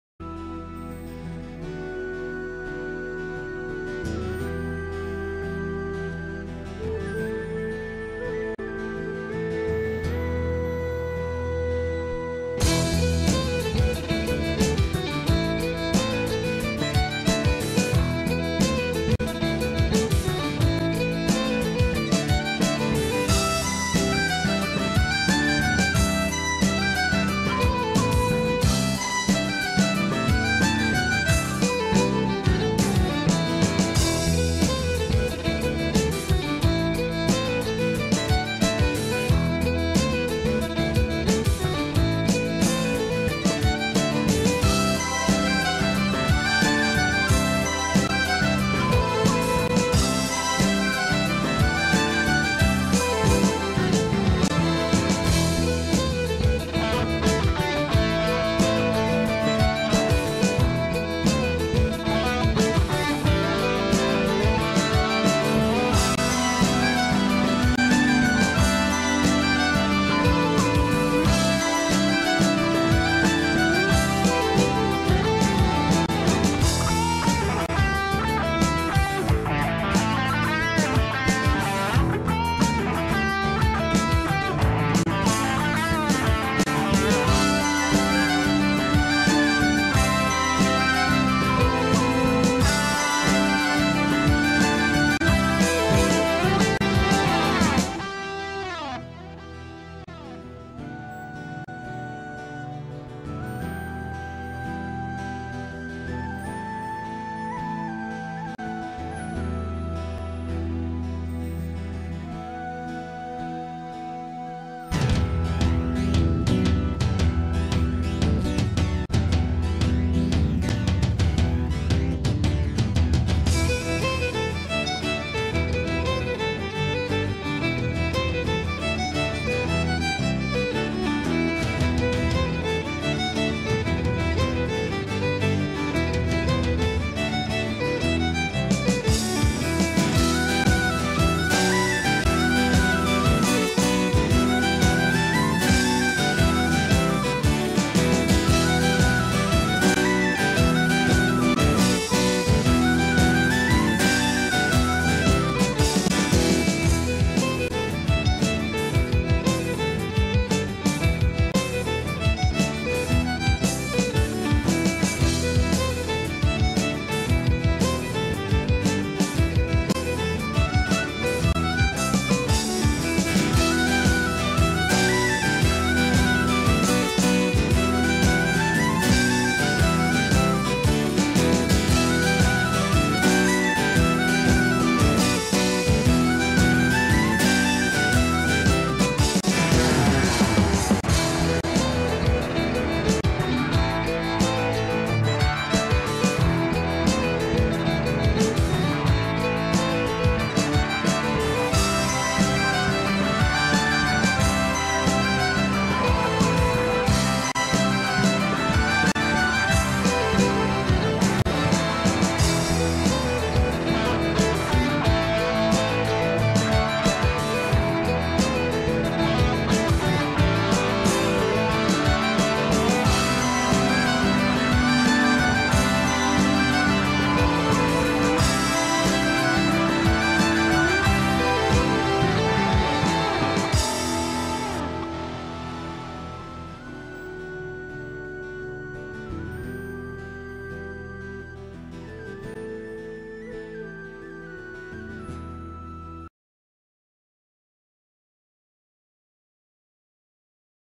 2013Australia Daypt1
Wangi's Australia Day regatta 2013. A huge day, big winds and a storm.